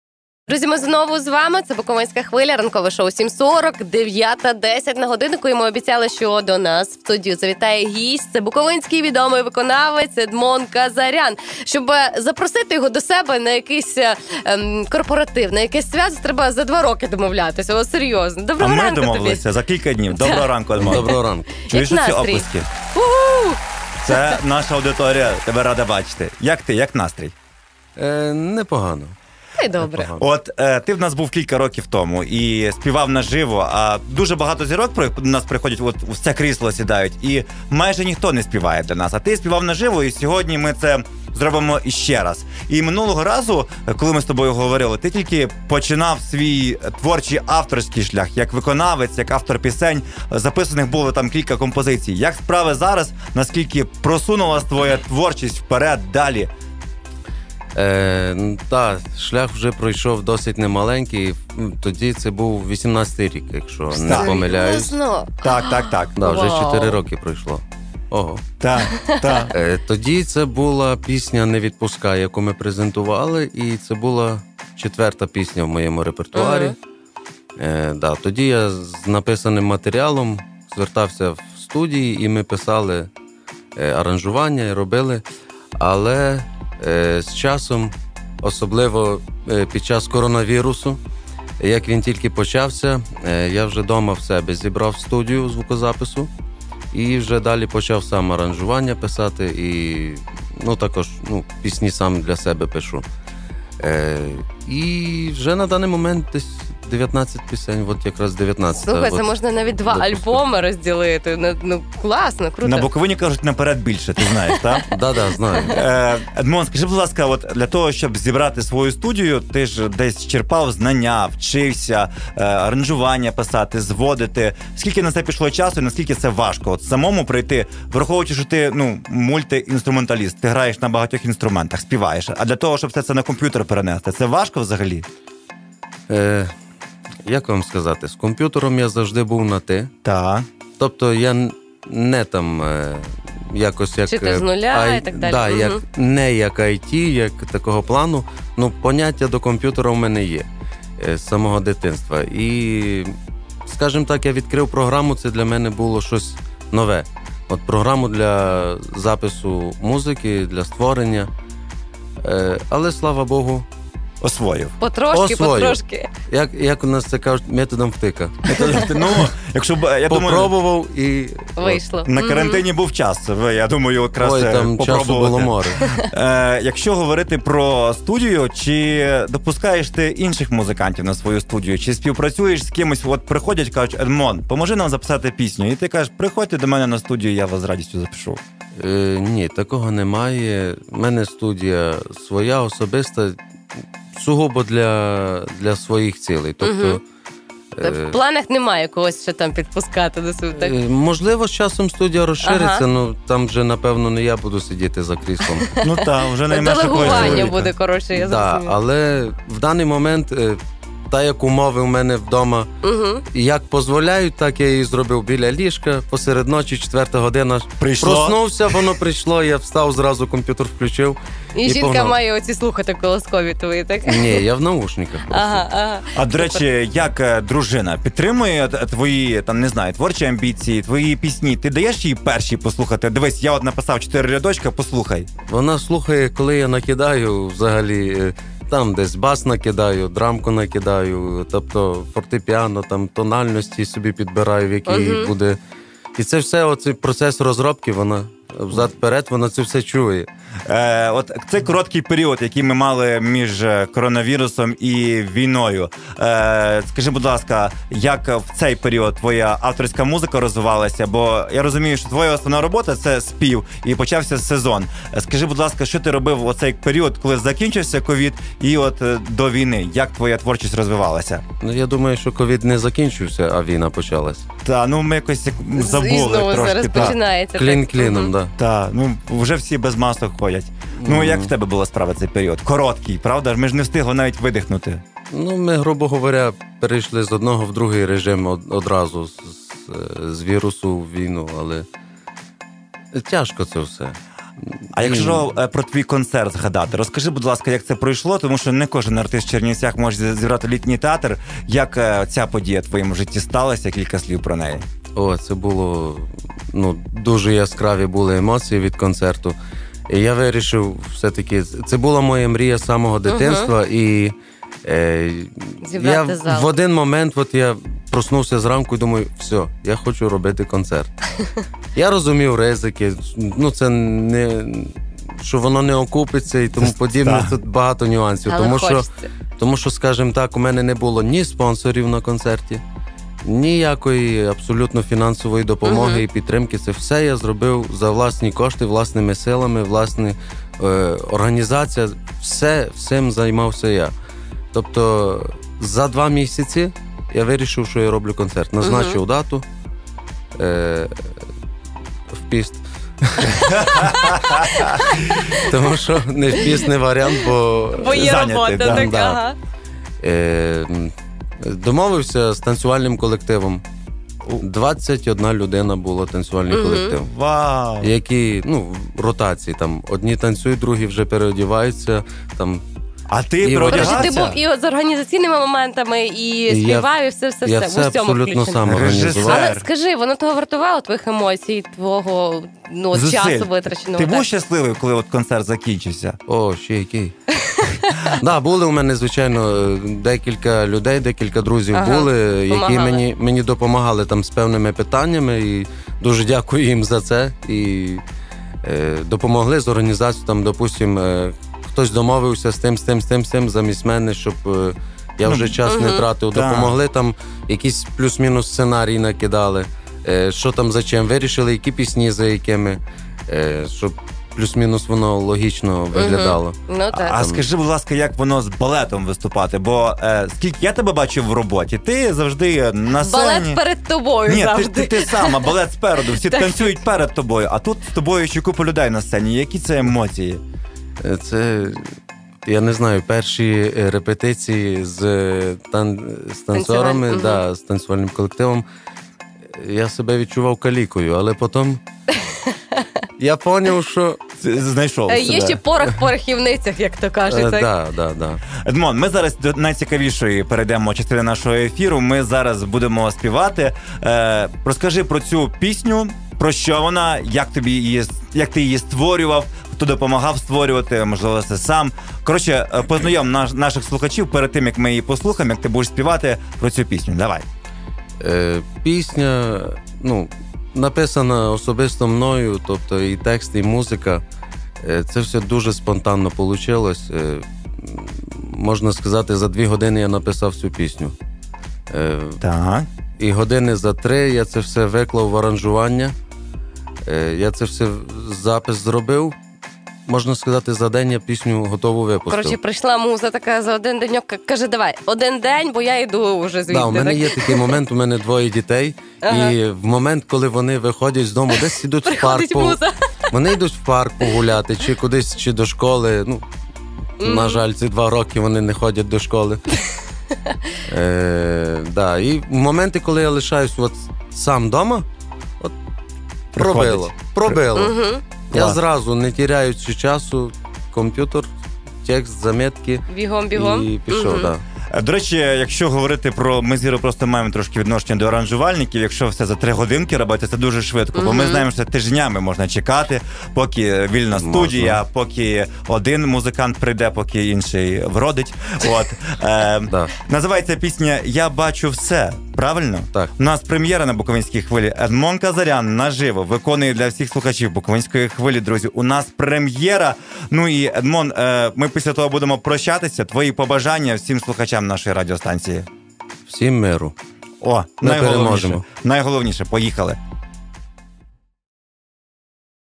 на Буковинській Хвилі
Відомий музикант Едмон Казарян розповів про свою авторську творчість. Поділився подробицями роботи під час карантину та війни. Повну розмову можете переглянути у відео нашого інтерв’ю.